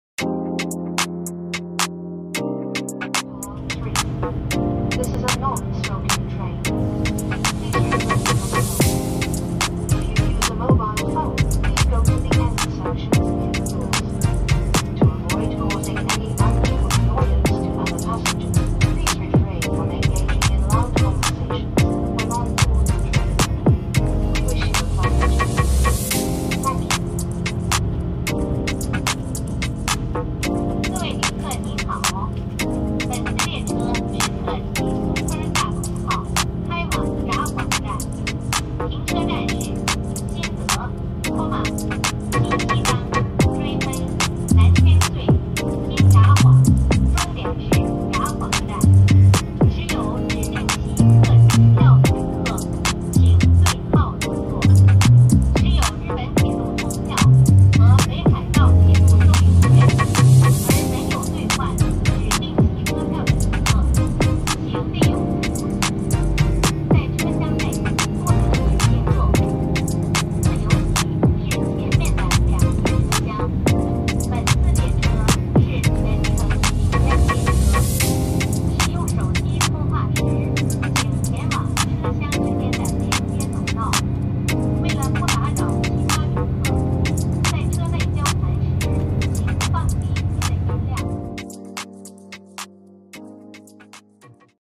【北海道風景】帯広貨物ターミナル
帯広貨物ターミナルを車窓から撮ってみました。